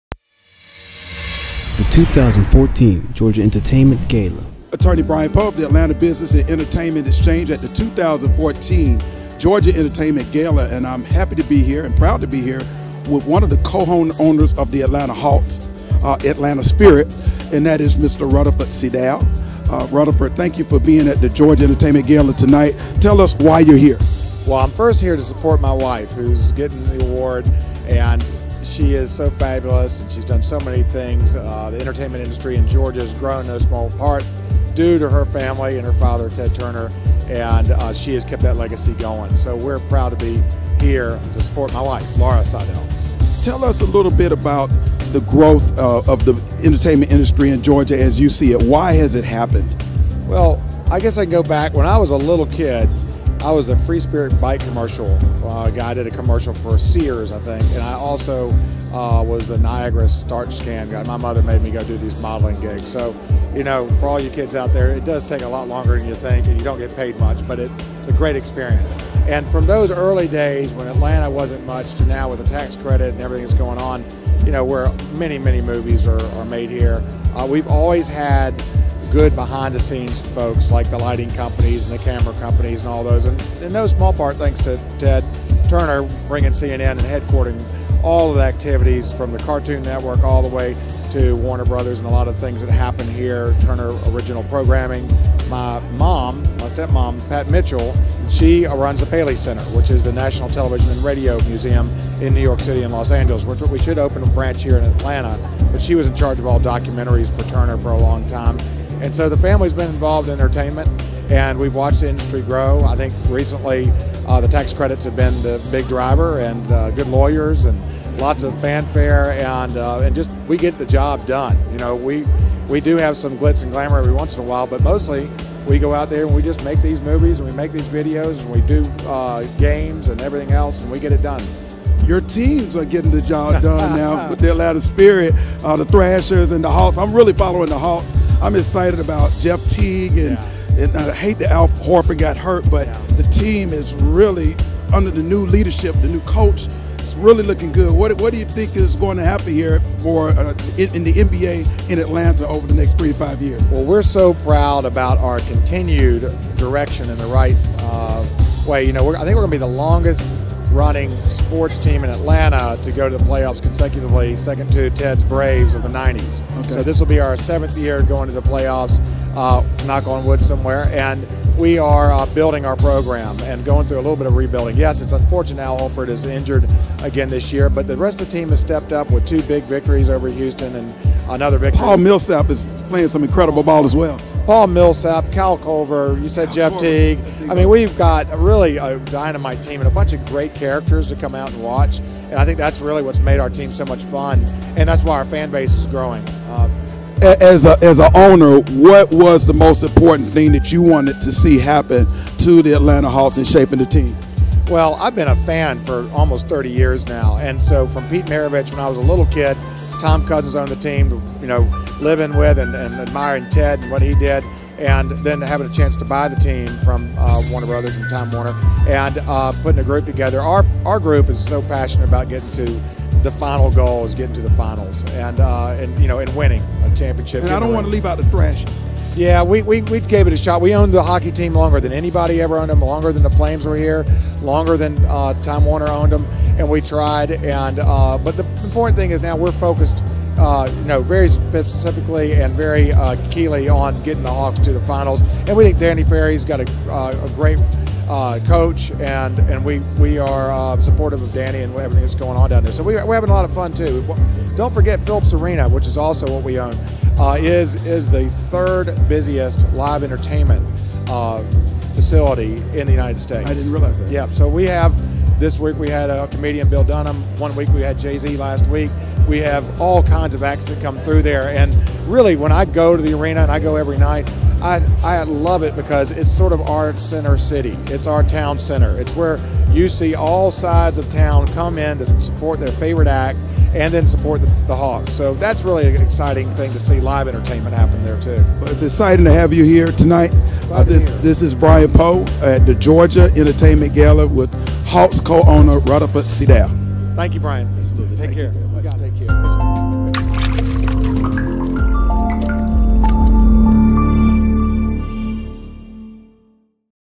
2014 GA Entertainment Gala - Rutherford Seydel (with Attorney Poe)
Rutherford Seydel is truly an icon in the community of Atlanta and this title extends across the globe reflecting the works and commitment of Rutherford.  As co-owner of the Atlanta Spirit (owners of the Atlanta Hawks and Phillips Arena), he spoke briefly with Attorney Poe at the 2014 Georgia Entertainment Gala (hosted by Autumn Bailey Ford).  We are proud to have produced this interview and video.  Daniel Fowler on behalf of the team at American Technologies.